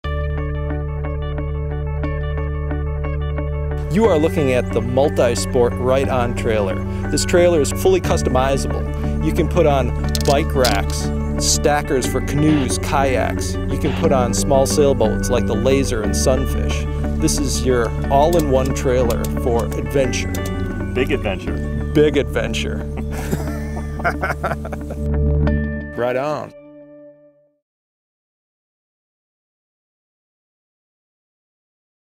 rightontrailerV1
Right-On Multi-Sport Trailer

The Right-On multi-sport trailer has round crossbars like the Yak® roof racks. Simply, pad the crossbars to carry surf boards, stand-up paddle boards, kayaks, fishing kayaks, or other small boats. The cross bars also accept Yak® and Thule® accessories to mount multiple bike racks, kayak stackers, cargo boxes and cages, and more. The user can configure the accessories to meet your needs while saving fuel from the reduced roof top windage.